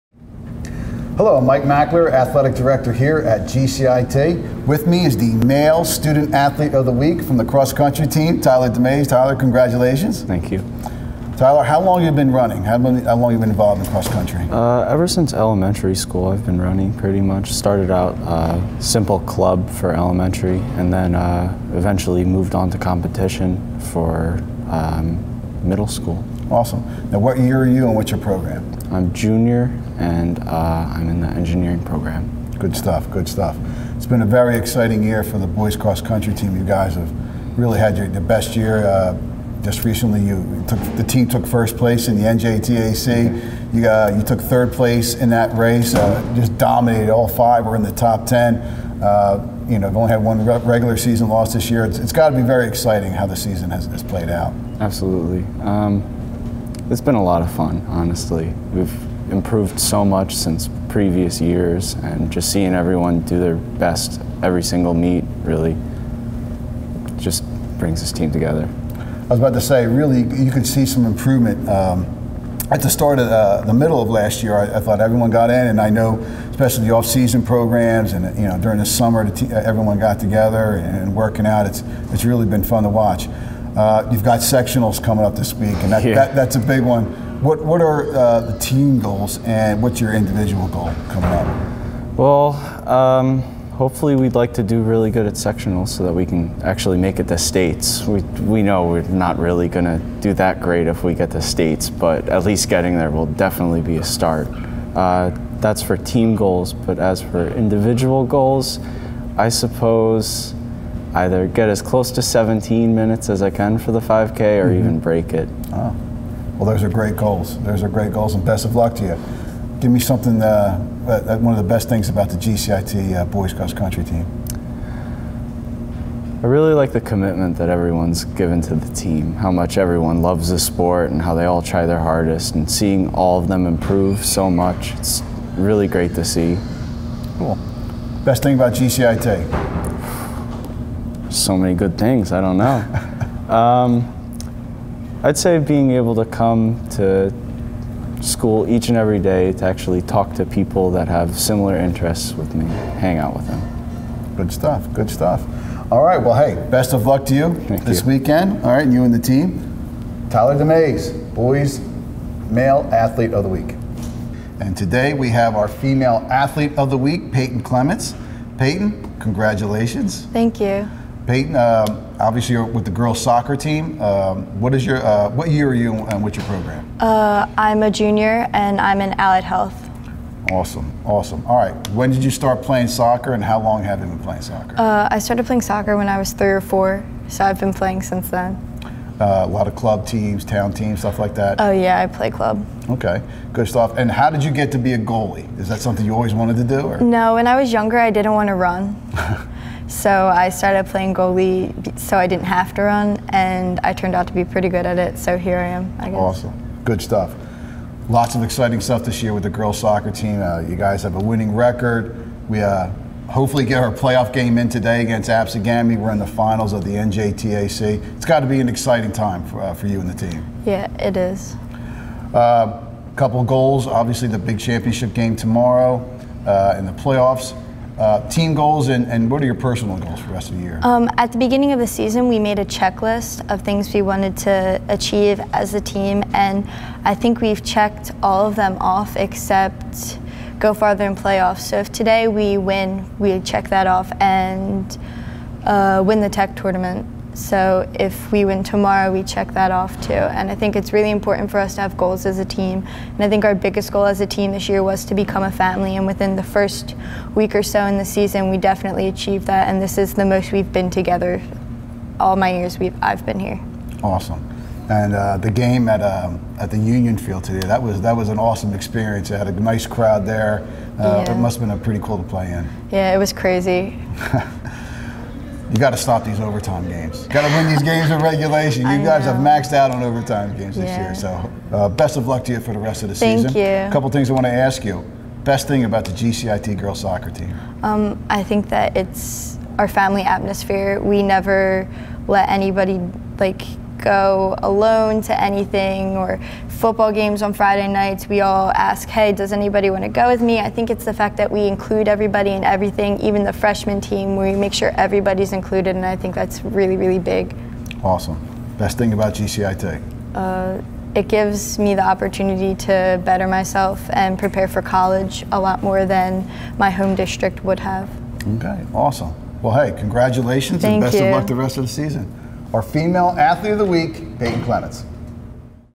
GCIT Featured Athletes November 2017
Tyler DeMaise, junior, on the GCIT Boys Cross Country team which captured its first ever NJTAC championship and a big reason for that title Tyler.  Tyler placed 3rd at the NJTAC Championship and ran an impressive time of under 18 minutes at the TCC Showcase.

Payton Clements, junior, on the GCIT Girls Soccer team is having a banner season and an important part of that success is the GoalKeeper, Payton. As a junior she has started every game in goal for the Cheetahs this season and has put together some impressive statistics: 123 saves, 10 shutouts and a goals against average of 0.9.